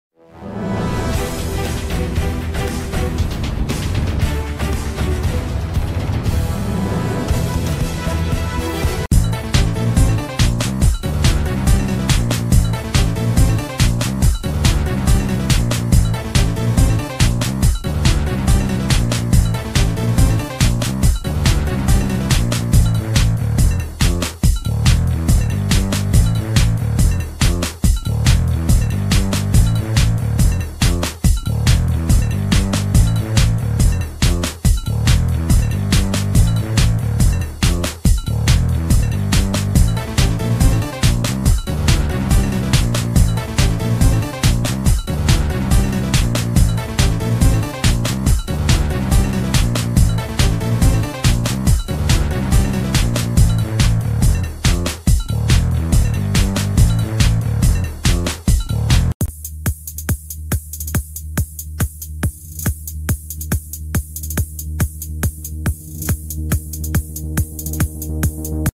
No better than Rashord: 59% of Man United fans don't want club to sign 62-goal PL striker
Many Thanks./